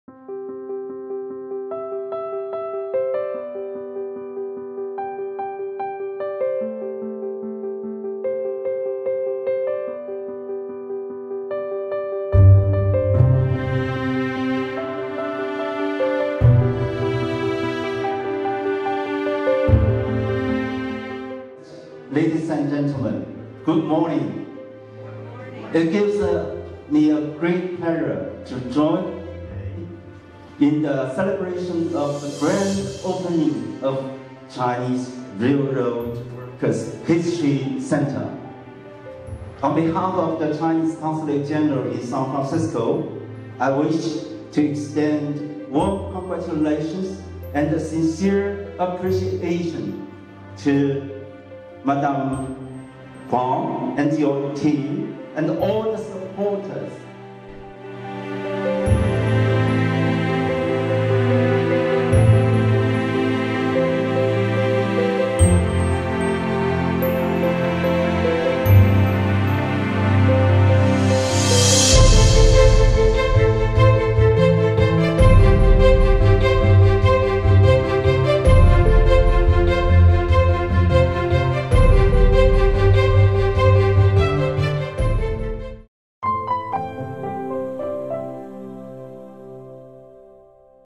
铭记历史！美国铁路华工中心在旧金山落成
今年是连接东西海岸的太平洋铁路建成154周年，当地时间5月10日中午，由旧金山华裔慈善家方李邦琴创建的美国铁路华工中心，在旧金山华埠剪彩落成。

这座由华裔慈善家方李邦琴创建的美国铁路华工中心于当地时间5月10日正式向民众开放。中国驻旧金山副总领事潘庆江，加州副州长库纳拉斯基(Eleni Kounalakis)，旧金山市议长佩斯金，以及湾区多座城市的华裔民选官员，出席了剪彩仪式。